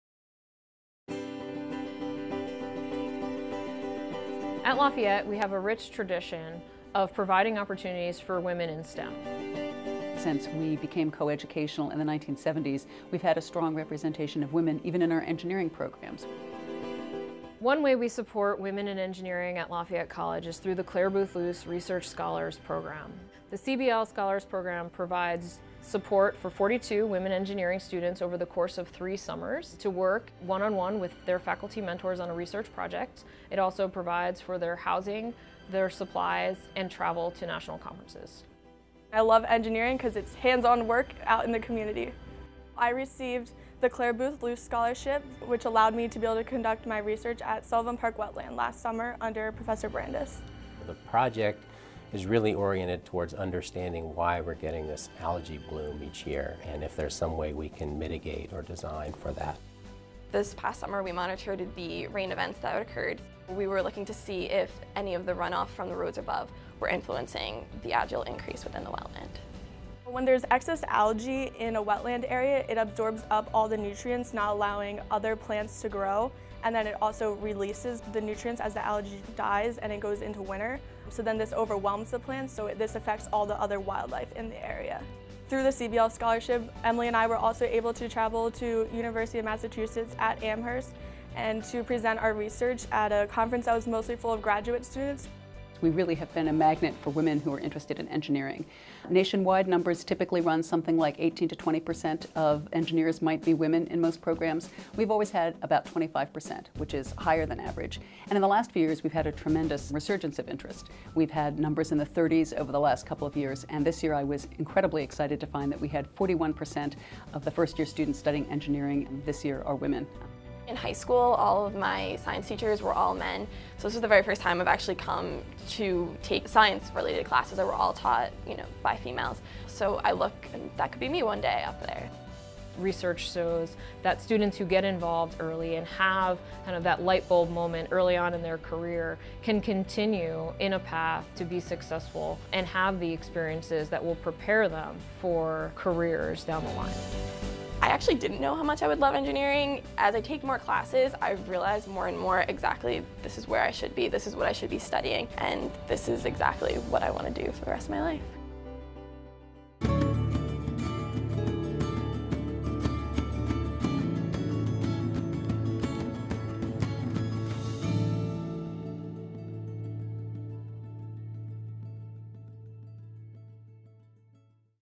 PBS39 and WISE Lafayette College
It’s WISEWednesday! Learn how @Lafayette College supports women in engineering through the Clare Boothe Luce Research Scholars program.

WISEWednesday is a co-production of PBS39 and the @Da Vinci Science Center Women in Science and Engineering (WISE) Initiative, with major funding provided by the Dexter F. and Dorothy H. Baker Foundation.